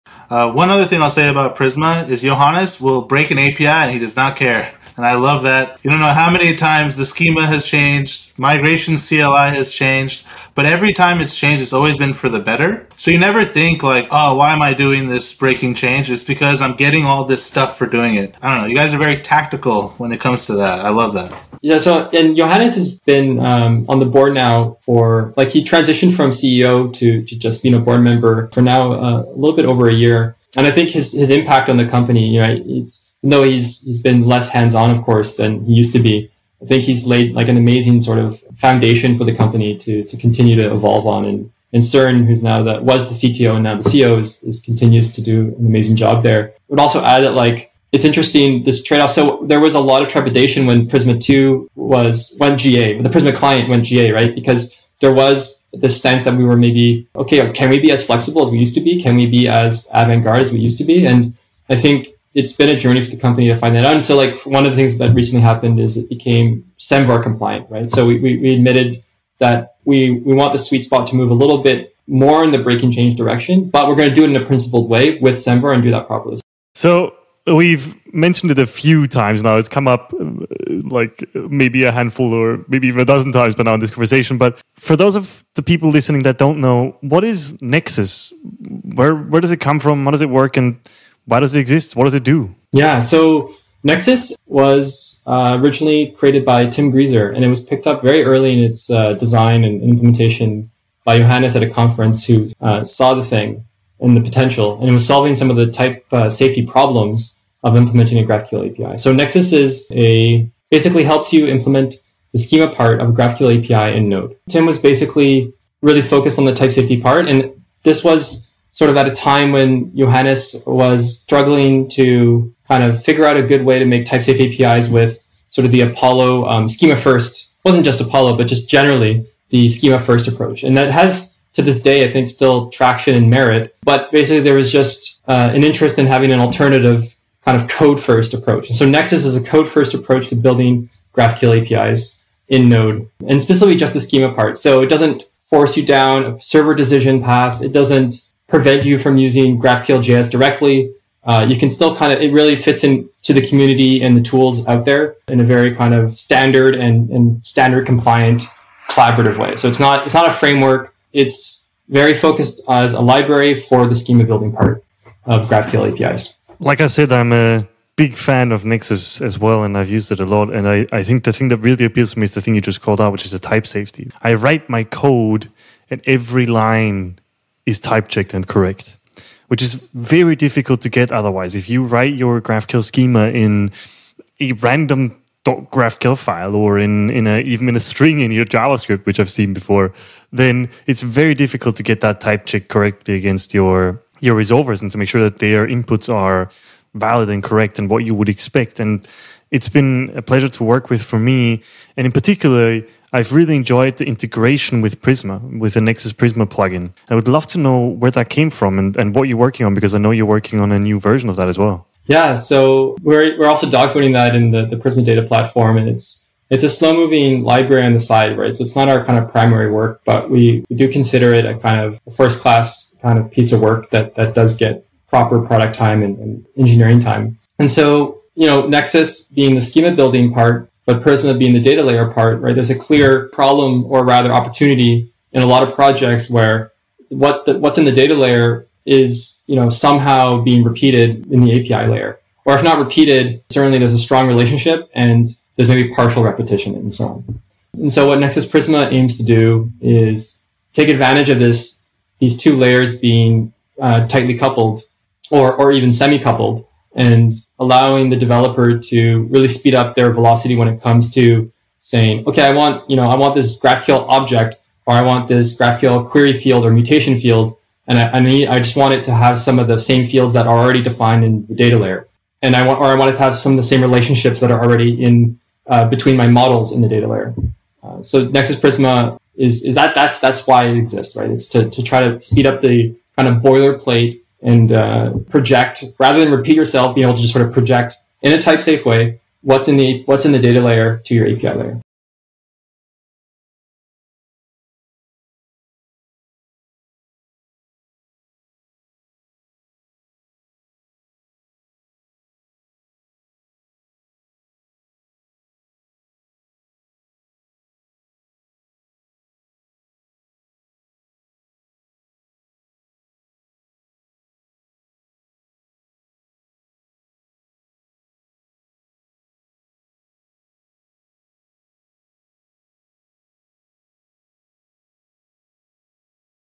Jason Kuhrt | Clip #05 | Prisma | Breaking APIs | Tactical Breakage | What Is Nexus Prisma? | Nex...
Jason Kuhrt is a software engineer and trained designer with an interest in art, design, engineering and humanities. Jason is currently working as the tech lead at Prisma, the popular open-source ORM for Node.js and TypeScript. He is also the co-creator of Nexus, a code-first type-safe GraphQL schema builder.